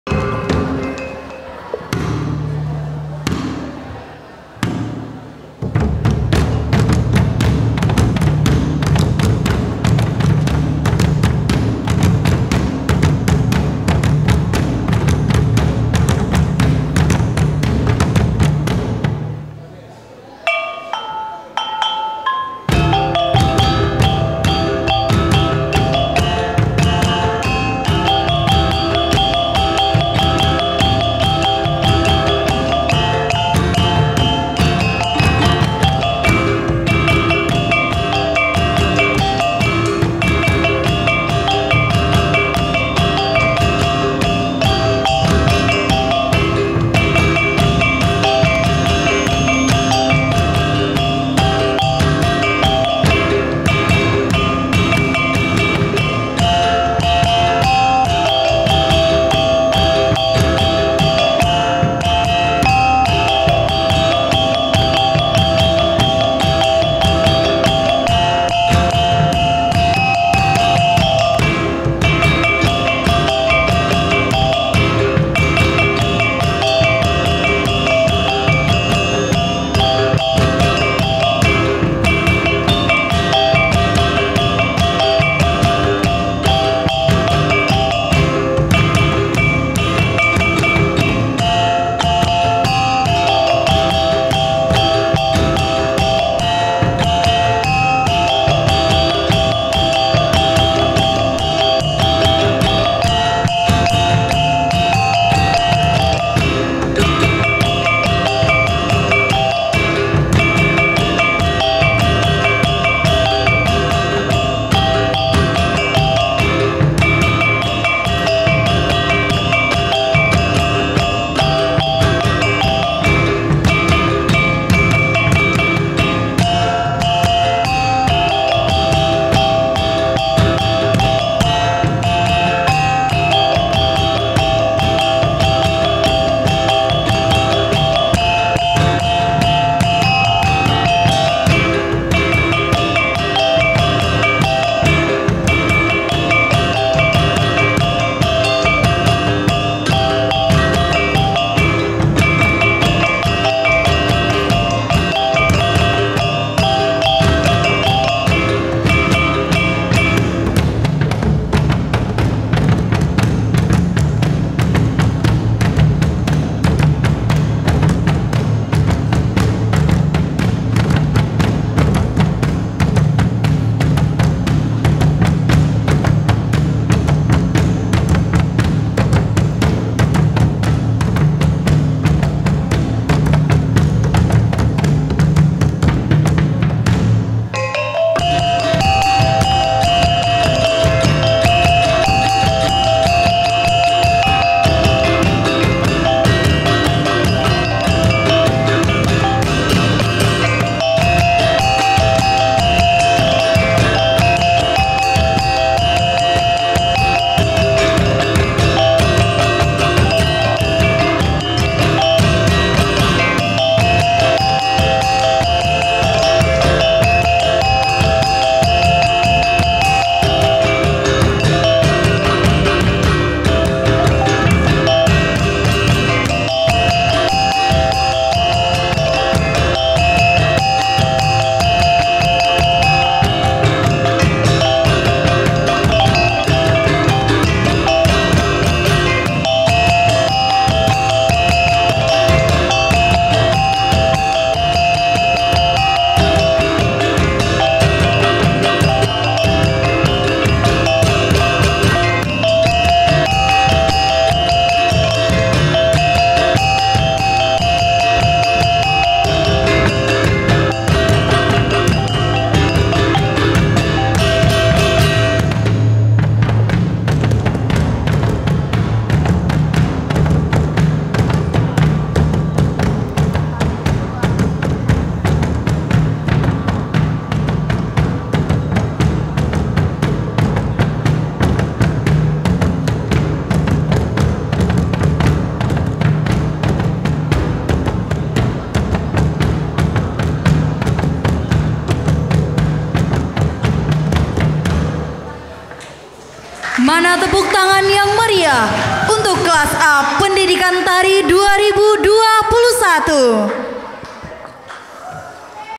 TARI RANTAK ⭕ UJIAN BASAMO ⭕ SENDRATASIK ™️ 2021 & 2023
UJIAN BASAMO (UAS MUSIK IRINGAN TARI & MUSIK TRADISI MINANGKABAU)

Januari - Juni 2024

Mata Kuliah :
- Musik Tradisi Minangkabau
- Musik Iringan Tari
- Praktik Instrumen Perkusi Tradisional

Mahasiswa Departemen Sendratasik Angkatan 2021 & 2023

Dosen : Uswatul Hakim & Hengki Armez Hidayat

Tempat : Medan Nan Balindung FBS UNP (Pendopo FBS)

Sound : Jonai, Bruno & Narji
Kamerawan : Mamek
Edit Video : J-Two Media  @JitumMedim

11 Juni 2024